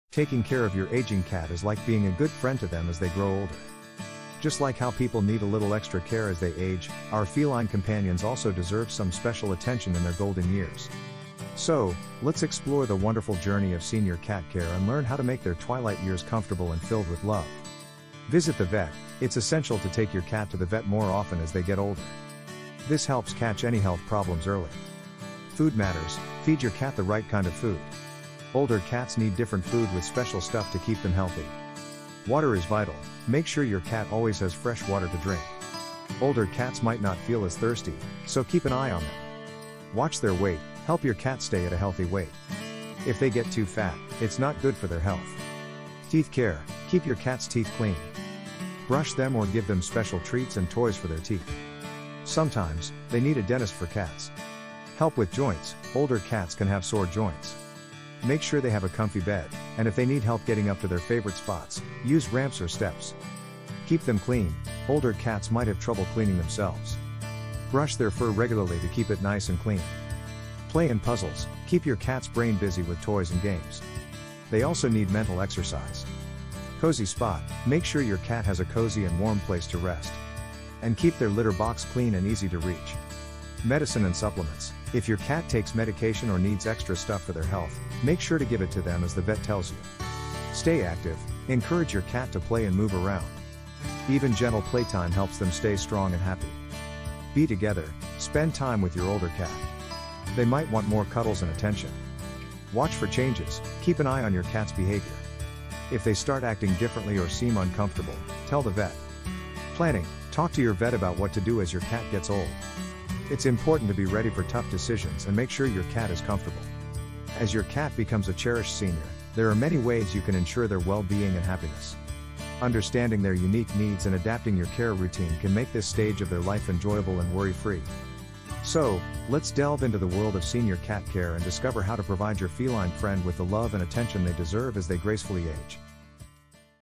Senior Cat Care 101! Essential Tips for a Smooth Transition!
In this series, we'll delve into the world of senior cat care, offering you invaluable insights and expert advice on how to support your aging cat's health and happiness. Learn how to recognize the signs of aging in your cat, from changes in behavior to physical transformations. Discover nine essential tips and strategies to ensure your senior cat enjoys their golden years to the fullest.
The Pet Connection